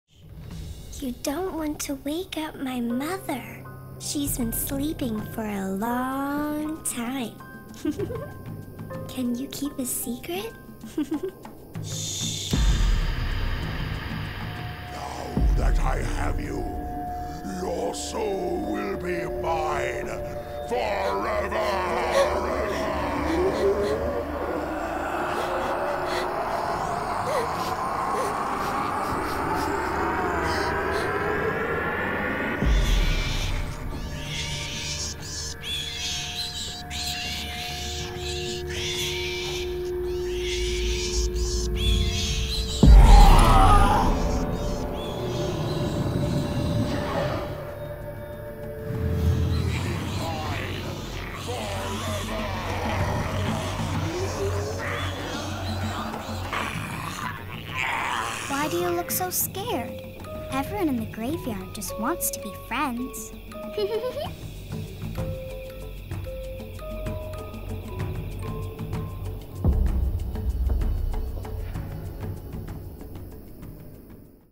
Reaper's Island Cave - Spirit Halloween
Explore the Reaper's Island Cave with all of its creepy creatures at your local SpiritHalloween!